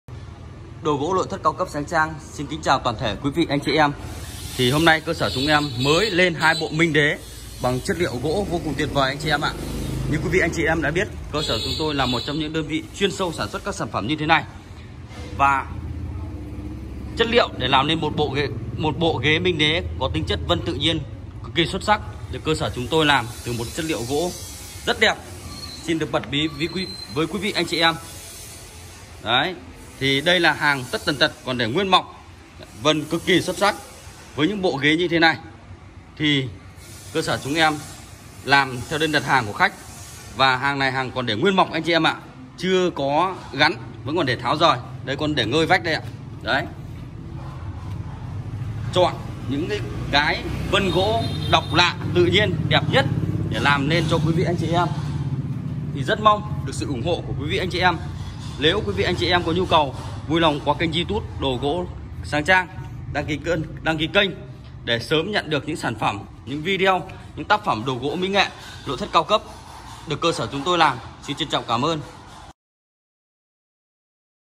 ✅ siêu phẩm minh đế gỗ sưa Nam Mỹ chọn Vân rất đẹp cho khách quý#minh đế_ghế hoàng cung#minh đế đẹp
✅ đồ gỗ cao cấp Sáng Trang xin giới thiệu tới quý khách hàng thân yêu bộ sản phẩm ghế minh đế được làm từ gỗ sưa Nam Mỹ quý hiếm , được cơ sở bên em chọn Vân Trần tự nhiên rất đẹp cho khách quý
✅ sản phẩm được làm theo phong cách nội thất nhà minh đơn giản nhưng đầy sự tinh tế
✅ đồ gỗ Sáng Trang có địa chỉ tại khu làng nghề đồ gỗ kim thiều- hương mạc- từ Sơn Bắc ninh